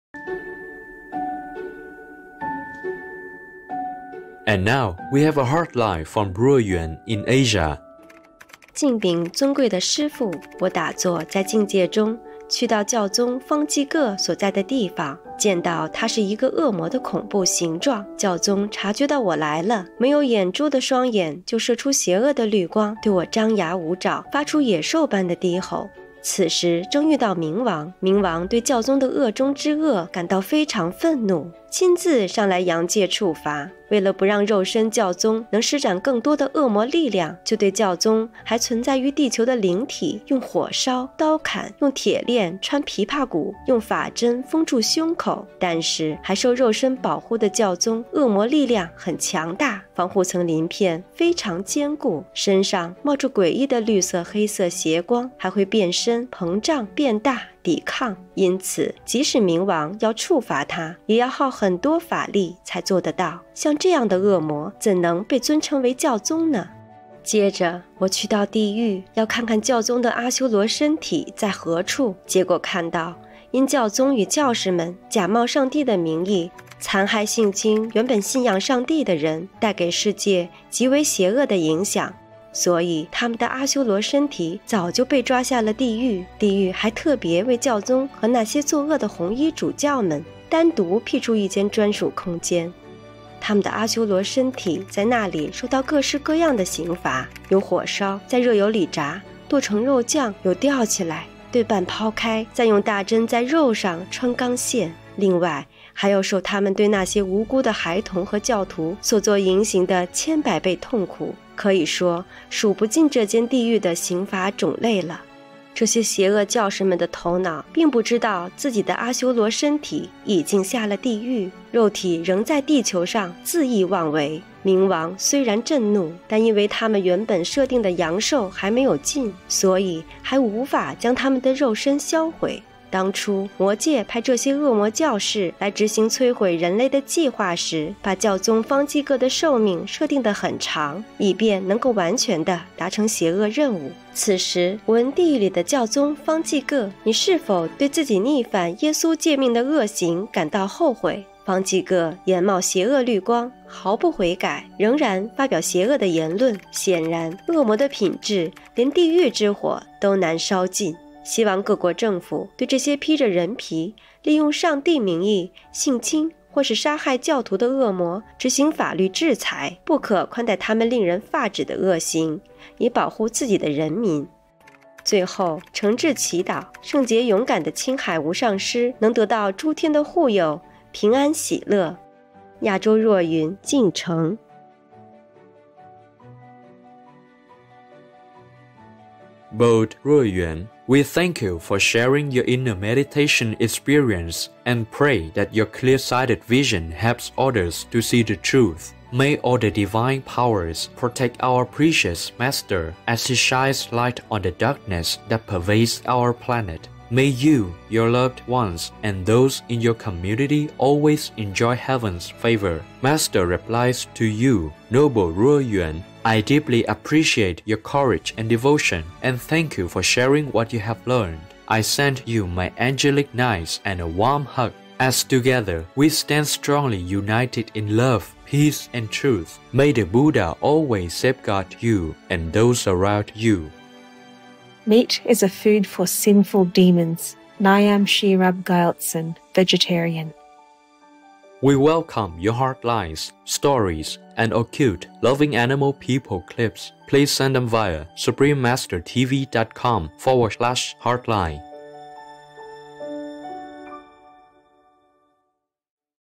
内なる体験: 悪魔がどうしてローマ法王として尊敬されるのだろう (Inner Experience How can such a devil be revered as the pope) JP
要約
To the Noble and Revered Master: During meditation, I went to the place where pope Francis was and saw that he was in the horrible form of a devil!
When the pope noticed my presence, his eyes, with no eyeballs, shot out vicious green light, he bared his fangs, brandished his claws, and growled like a wild beast!
At that time, the king of Hades appeared. Furious with the pope’s most evil deeds, he came to this world to punish the pope. To stop the pope, in a human body, from using more demonic power, the king of Hades burned and cut the pope’s astral body on Earth, threaded his collarbones with iron chains, and sealed his chest with magic needles.
However, protected by this physical body, the pope’s vicious power is still very strong, and the protective layers of scales are solid. His body emitted weird and wicked green and black light. He could transform into different forms, and expand his body to fight back. It took a lot of power for the king of Hades to punish the pope.
How can such a devil be revered as the pope?
After that, I went to hell to look for the Asura body of the pope and found that, since the pope and his priests made use of the name of God to abuse and sexually assault the believers of God, causing evil and wicked influences to the world, their Asura bodies had long been captured and sent to hell. The hell even had an exclusive area for the pope and those evil cardinals.
Their Asura bodies are in hell suffering from all kinds of punishment, including being burned in fire, deep fried in hot oil, and ground into minced meat. Some were hung up, cut into half, and their flesh was pierced through with steel thread. Besides all of this, they still have to bear a thousand times more of the pain of the innocent children and believers who have been sexually assaulted by them. It’s impossible to count the ways of punishment in hell.
The minds of these evil priests are not aware their Asura bodies have gone to hell and they still behave recklessly on Earth. The king of Hades is furious but can’t destroy their physical bodies as their predestined lifespan has not ended yet.
When the devil’s world sent these demonic priests to execute their plan of destroying humankind, they gave pope Francis a long lifespan to accomplish their evil plan completely. “Do you feel repentant for your evil deeds which are against Jesus’ commandments?” I asked pope Francis in hell.
His eyes emitted vicious green light, he showed no signs of regret, and still spoke loudly of his evil views. Apparently, even hell’s fire is unable to cleanse his devilish quality.
I hope that the governments in the world will impose legal punishment on these devils in human skins, who use God’s name to sexually assault innocent people and kill God’s believers, and will not tolerate their outrageously evil deeds, to protect their own people.
Lastly, I sincerely pray that the Divine, Pure and Courageous Supreme Master Ching Hai be blessed and protected by all Heavens and be in peace, safety, joyfulness, and happiness. Respectfully, Jo-Yun in Asia

Bold Jo-Yun, We thank you for sharing your inner meditation experience and pray that your clear-sighted vision helps others to see this truth. May all the Divine powers protect our precious Master as She shines Light on the darkness that pervades our planet. May you, your loved ones, and those in your community always enjoy Heaven’s favor.
Master replies to you: “Noble Jo-Yun, I deeply appreciate your courage and devotion, and thank you for sharing what you have learned. I send you my angelic knights and a warm hug, as together we stand strongly united in Love, peace, and truth. May the Buddha always safeguard you and those around you.”